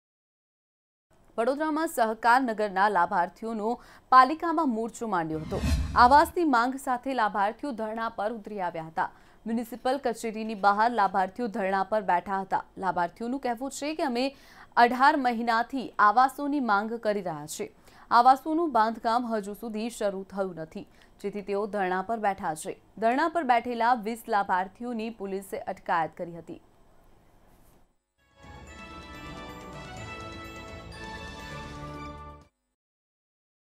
Vadodara: People sit on strike for the demand of houses of Awas Yojana | Mantavya News
Vadodara: People sit on strike for the demand of houses of Awas Yojana